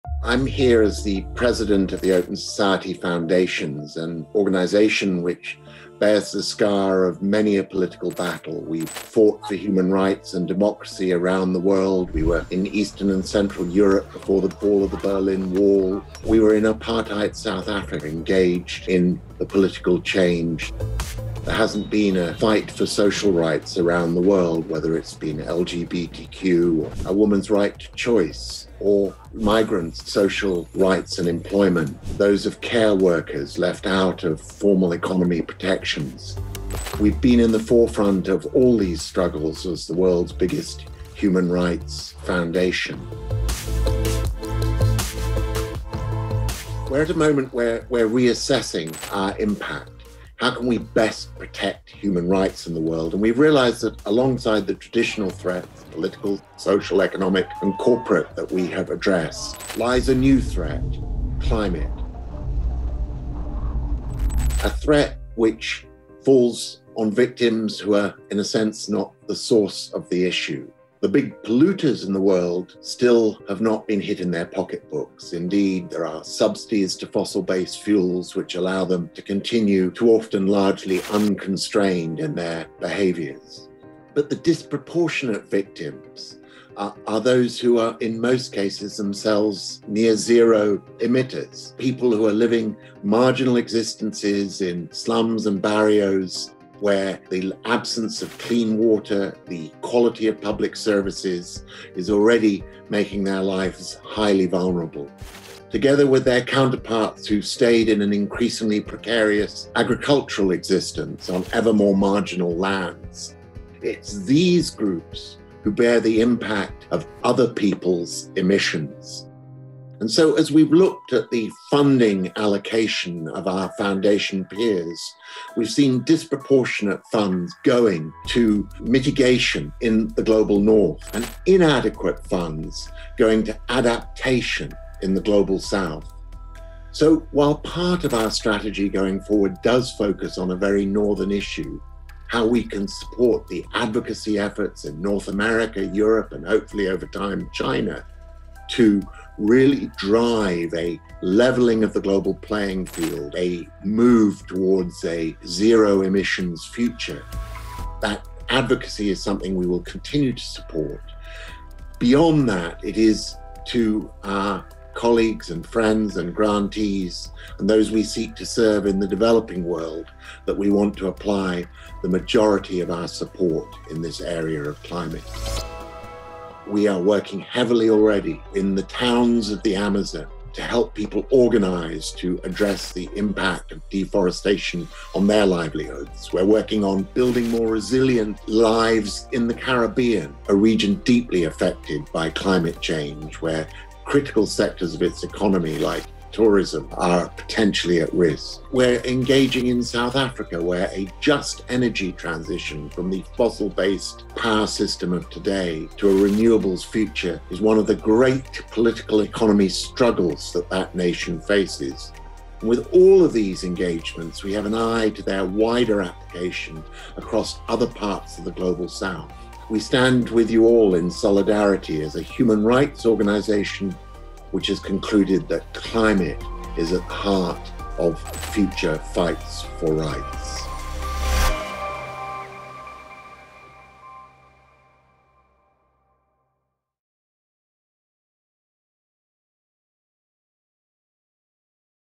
Mark Malloch-Brown at New Summits (Full Remarks)
Mark Malloch-Brown discusses the Open Society Foundations’ plans to support climate adaptation in the Global South in order to level the global climate action playing field.

For more PS, join the conversation on...